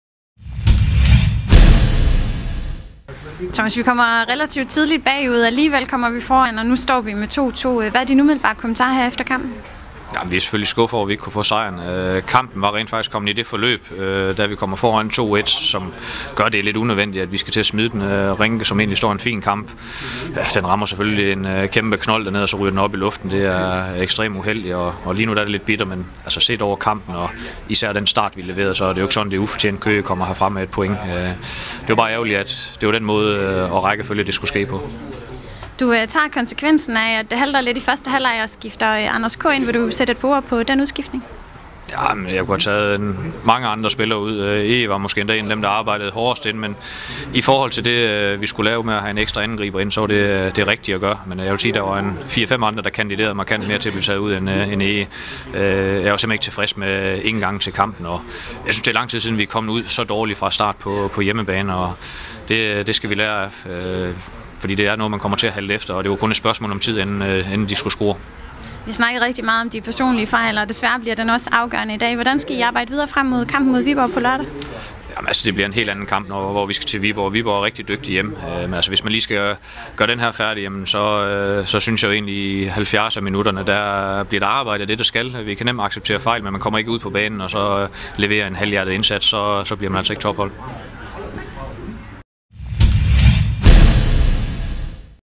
FCF-TV - Reaktion Thomas Thomasberg, FCF - HBK
Reaktion fra Thomas Thomasberg efter dagens uafgjorte resultat mod HB Køge.